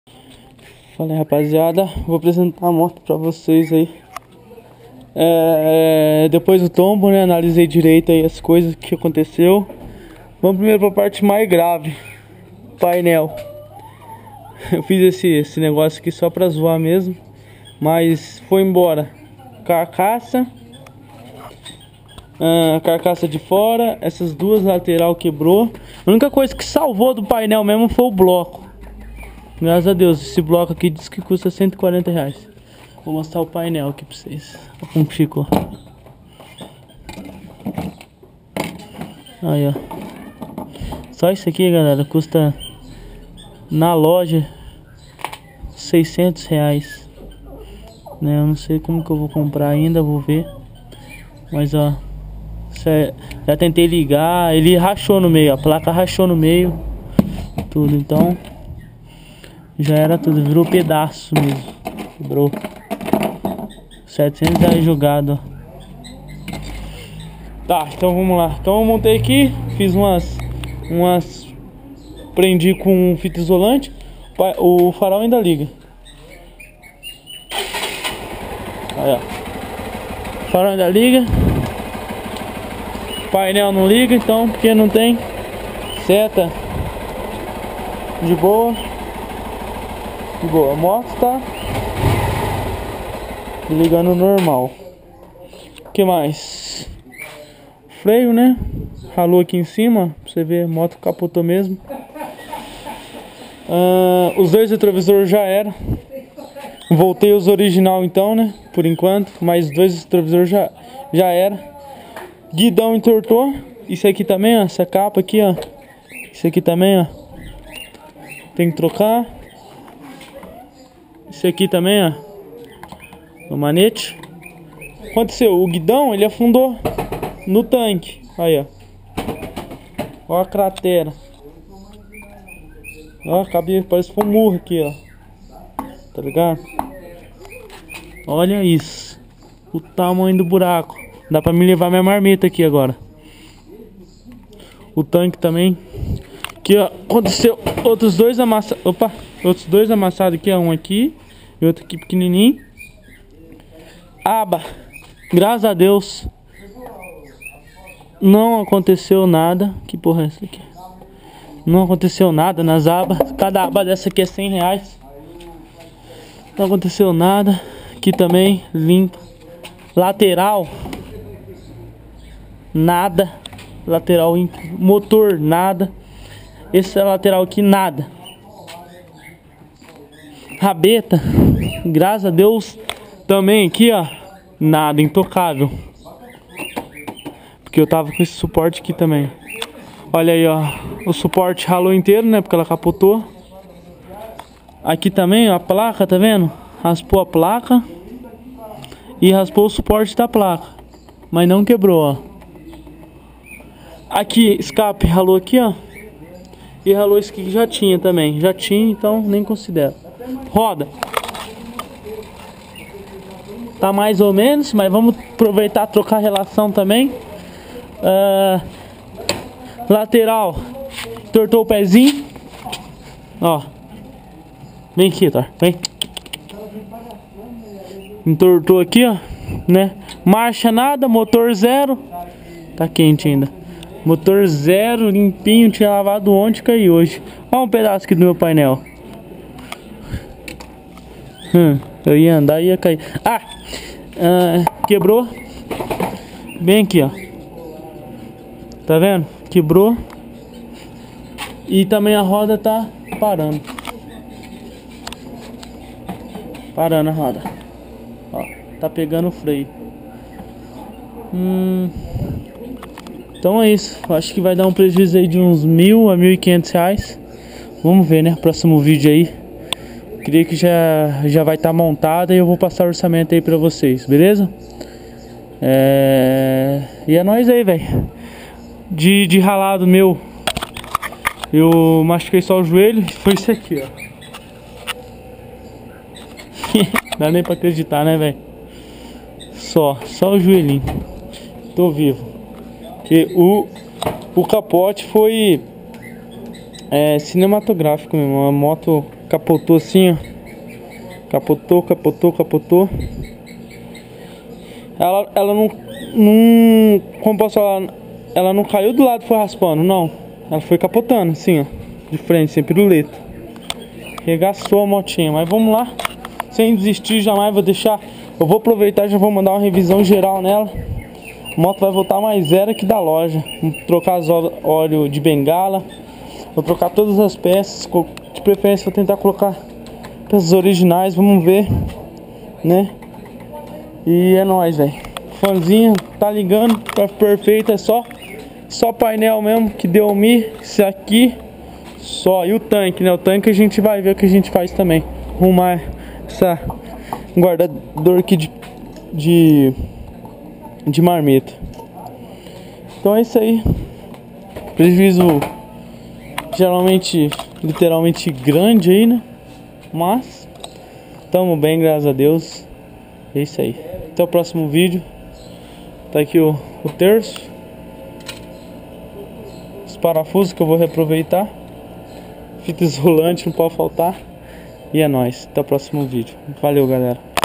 Mostrando o estado da MOTO depois do ACIDENTE
Fala galera, aqui eu já tava em casa , ai resolvi mostrar tudo sobre a moto ,depois de ter analisado tudo !!  FODA GALERA !! MEU SALDO NO BANCO FOI PRA 0 !!
É NOIS !!!
Moto : FAN 150cc 2014 PRETA ESDI
Câmera : Gopro Hero 2
Cidade : Londrina - Paraná
Motovlog Londrina